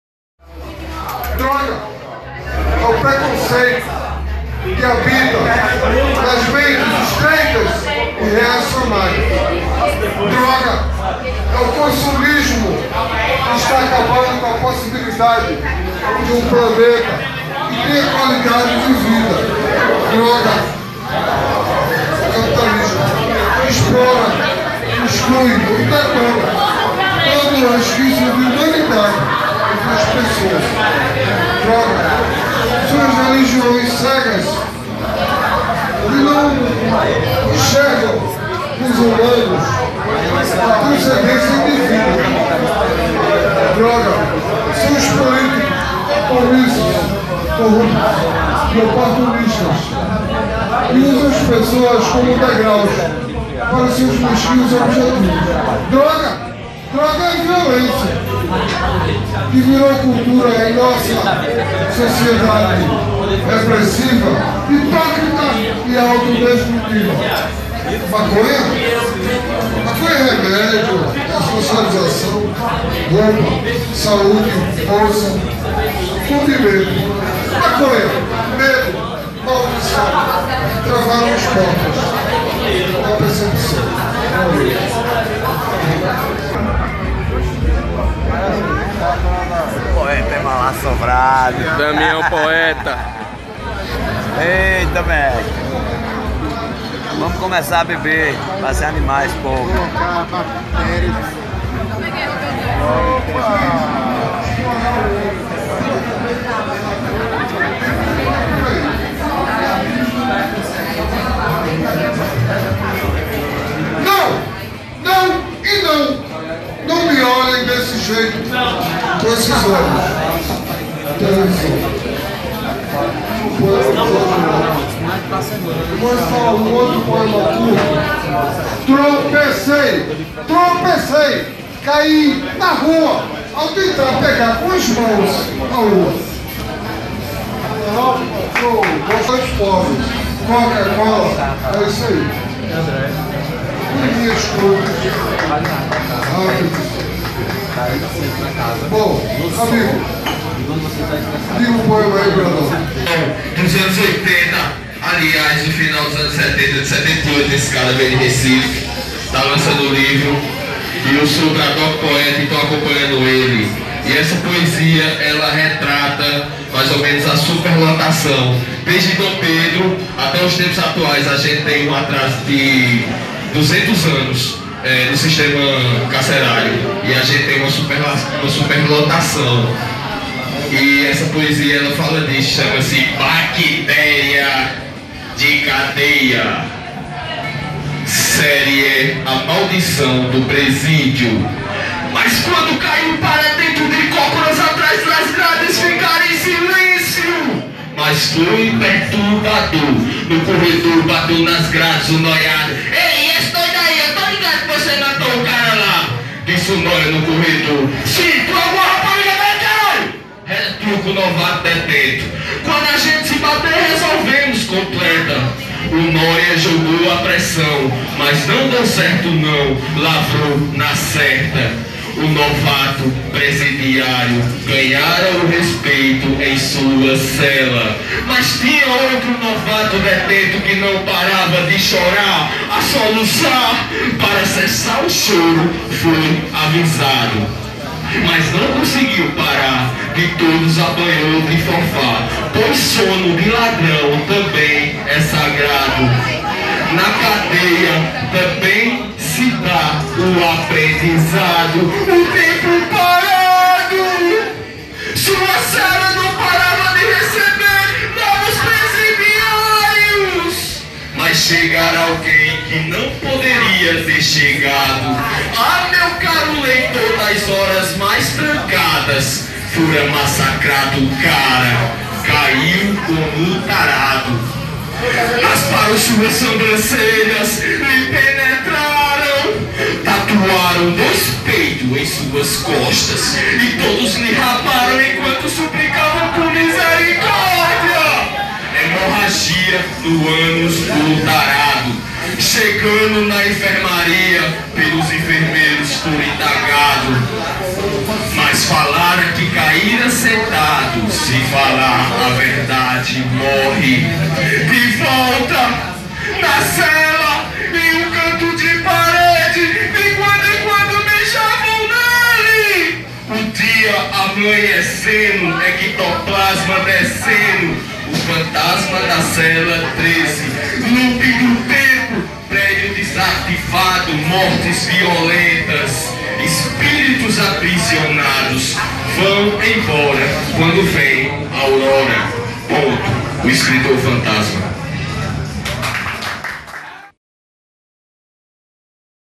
RECITAL BAR DO CARVÃO: Valmir Jordão & Bradock poeta ( Rua da má fama )
Valmir Jordão ( poeta ) Damião ( poeta )& Iram F. R. Bradock ( Poeta)
ORDEM CULTURAL DO AGRESTE
( Geração mandacaru )